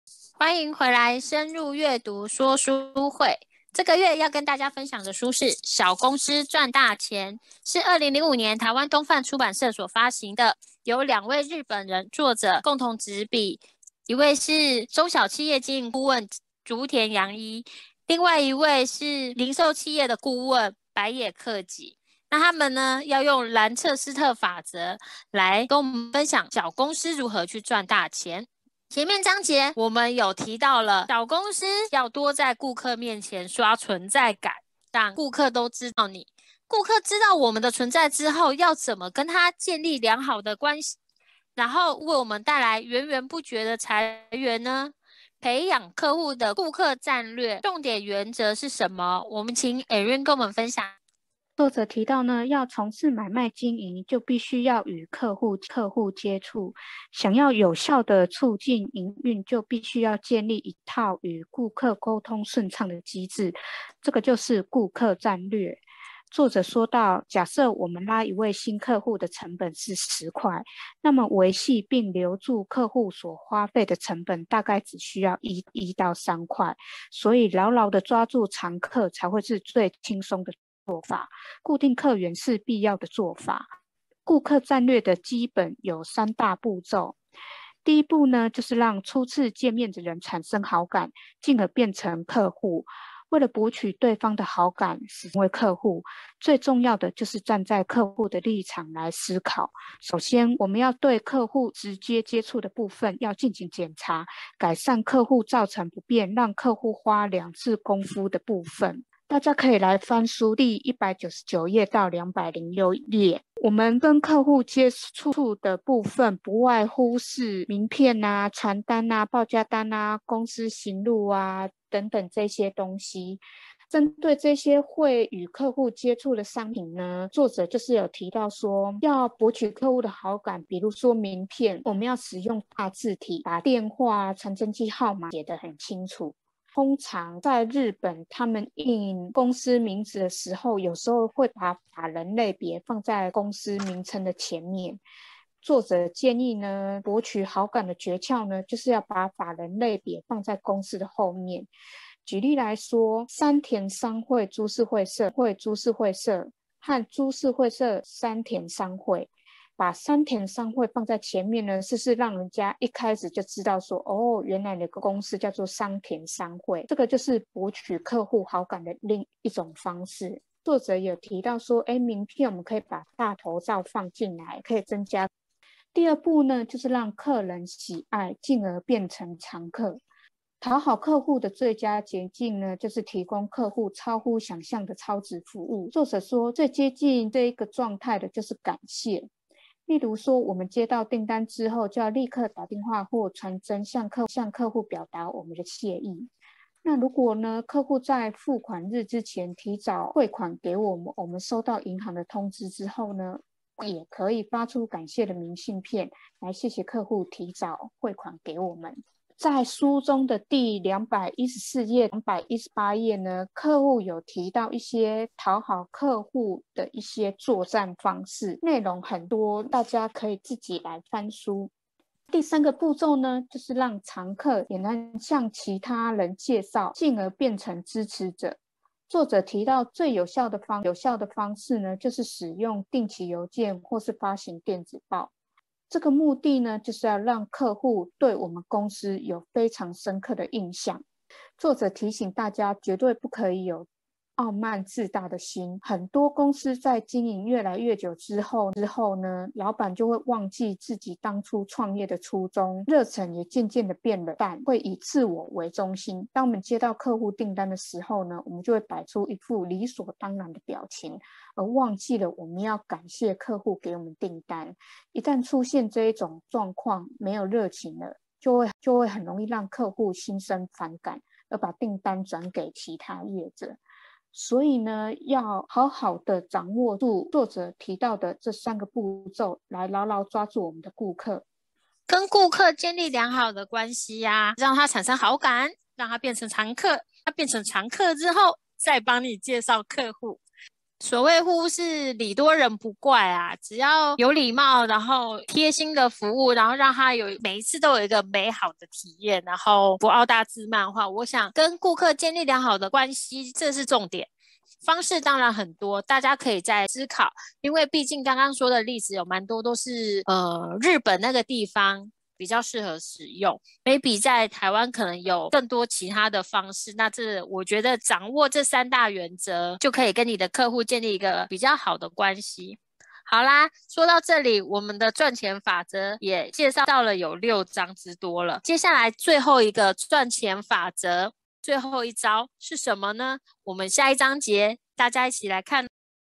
７．如何培養顧客－小公司賺大錢
本影片內容取自賺錢法則六
書名：小公司賺大錢
作者：竹田陽一、柏野克已
出版社：台灣東販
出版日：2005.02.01